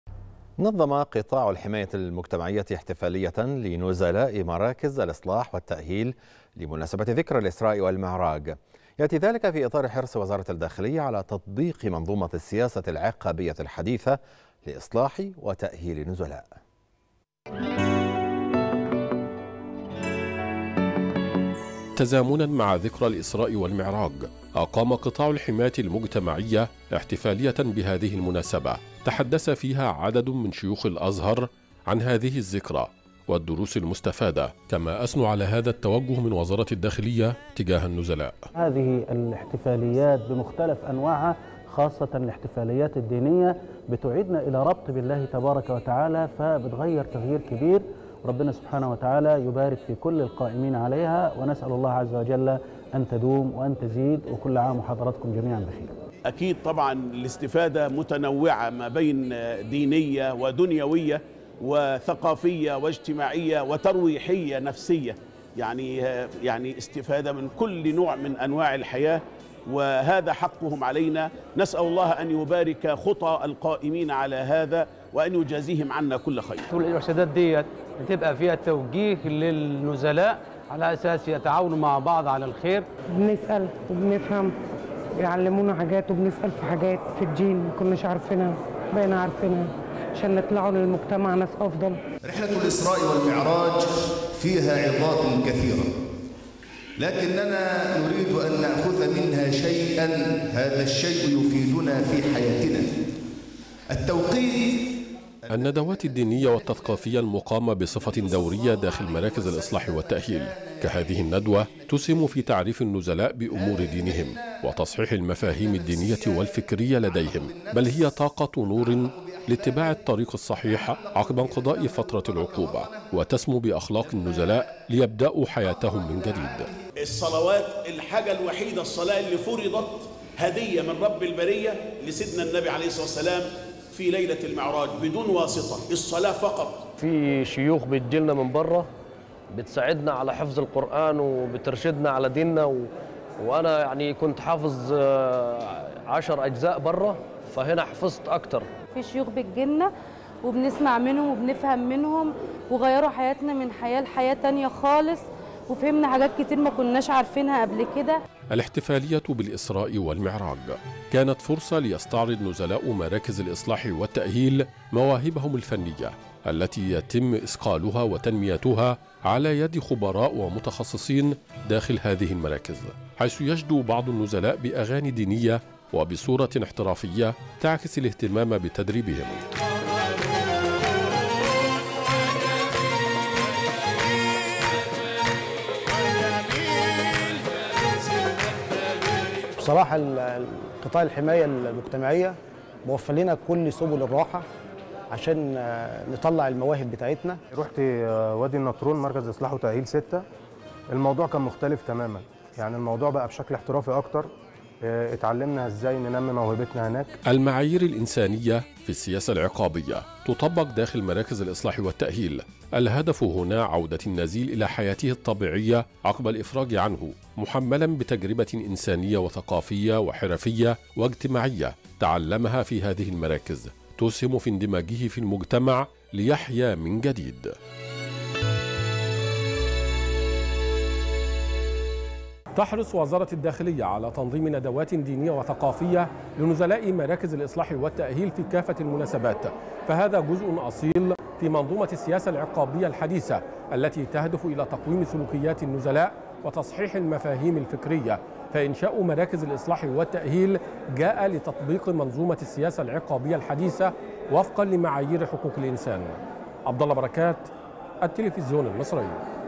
قطاع الحماية المجتمعية ينظم احتفالية لنزلاء مراكز الإصلاح والتأهيل بمناسبة ذكرى الإسراء والمعراج
نشرة التاسعة | قطاع الحماية المجتمعية ينظم احتفالية لنزلاء مراكز الإصلاح والتأهيل بمناسبة ذكرى الإسراء والمعراج